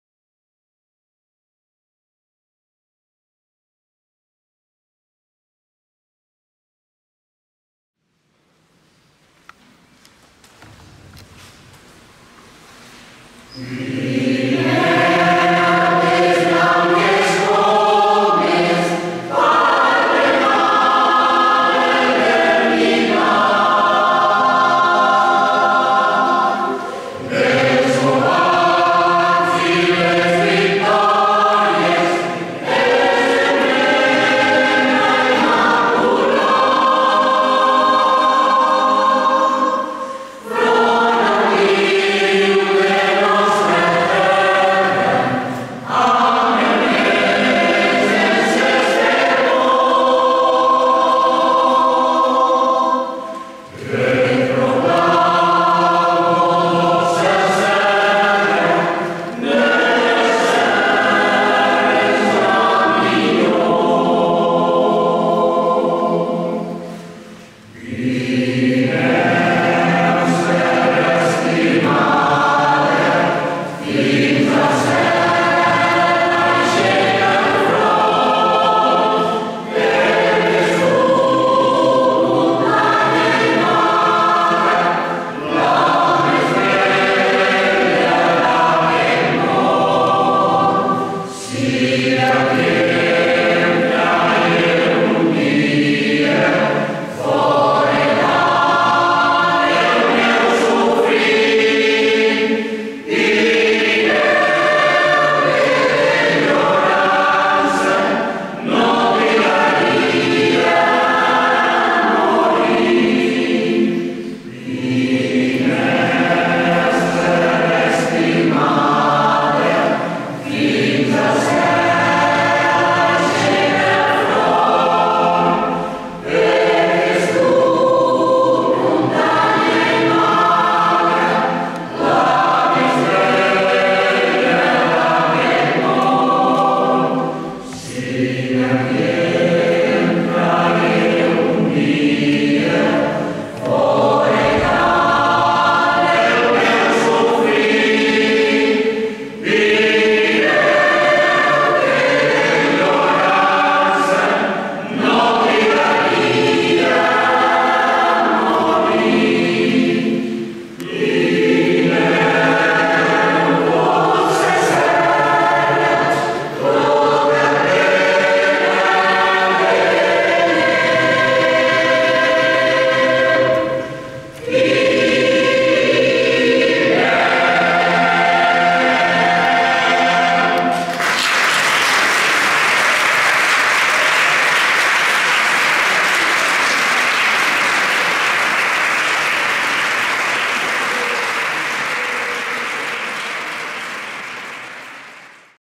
SEGRIÀ CORALS, CORAL BELL-CAMP ALGUAIRE 2017 Dir.: Robert Faltus
12è SEGRIÀ CORALS, CORAL BELL-CAMP, Director: Robert Faltus ALGUAIRE 2017,,,150è aniversari del naixement del pianista i compositor lleidatà Enric Granados (Lleida, 27 de juliol de 1867 – Canal de la Mànega, 24 de març de 1916)